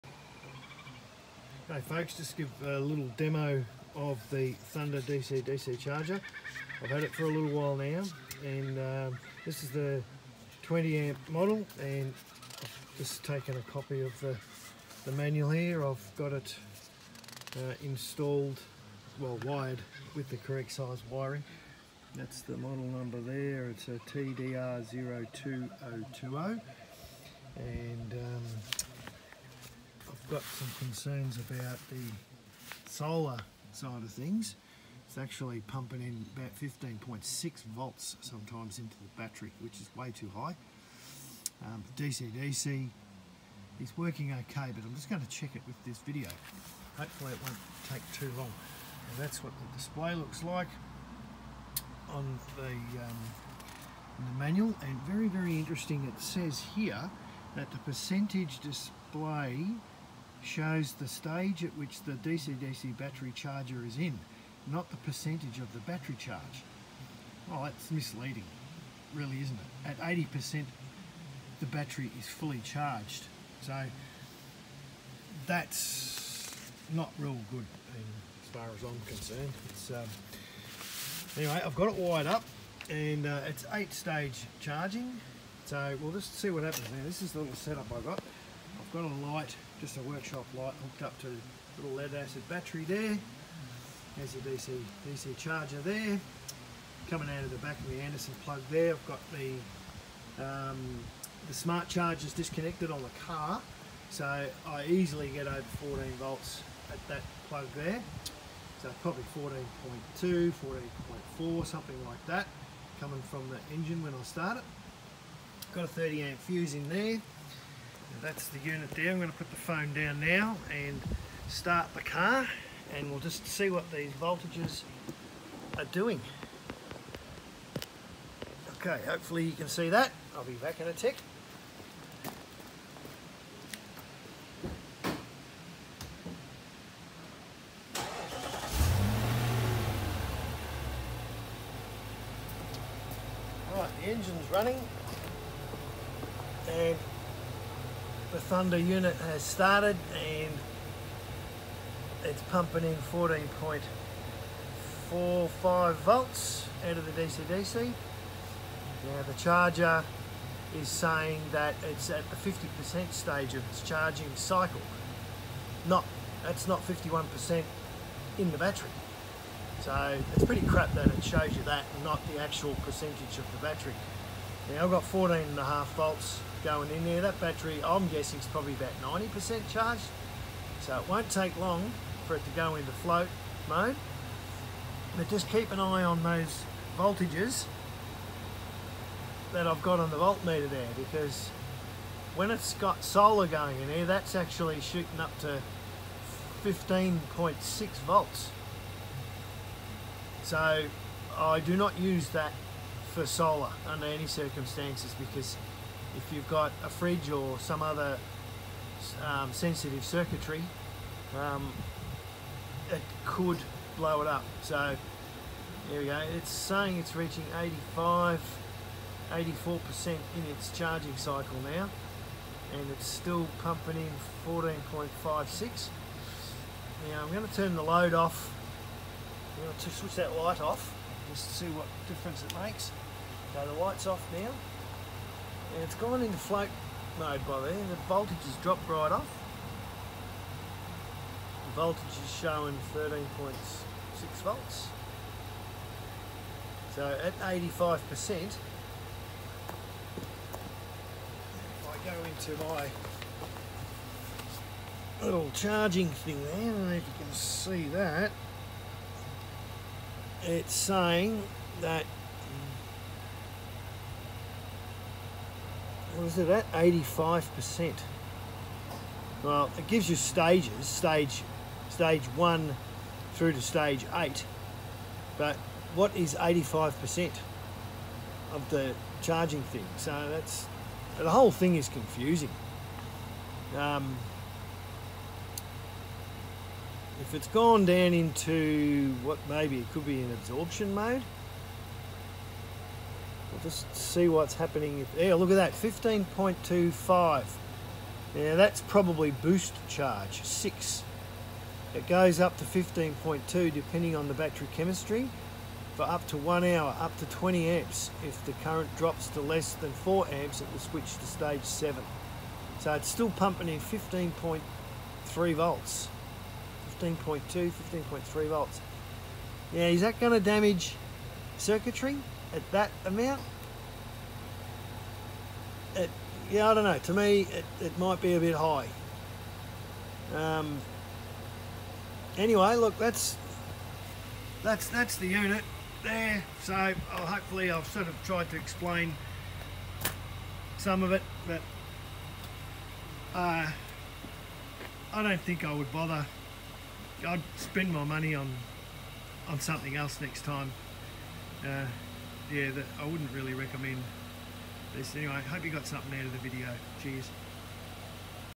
Thunder DC-DC charger test review
Testing the operation of the Thunder DC to DC charger on my car. It highlights how useless the display is and the potential for overcharging with high voltage output of the unit.
The Solar regulator part of it, in my opinion, is dangerous to use with sensitive circuits such as with portable fridges and the like due to the voltage going above 15.5v and at times as high as 15.7v.
In the manual, the “Boost” stage specifies 15.2v for short periods. Not sure if this is too high or not. The DC/DC operation from the car’s alternator seems to be ok but for the Boost stage.
I wouldn’t recommend this unit mostly due the the poor display properties. You get what you pay for I guess. This was pretty cheap compared to well known brands.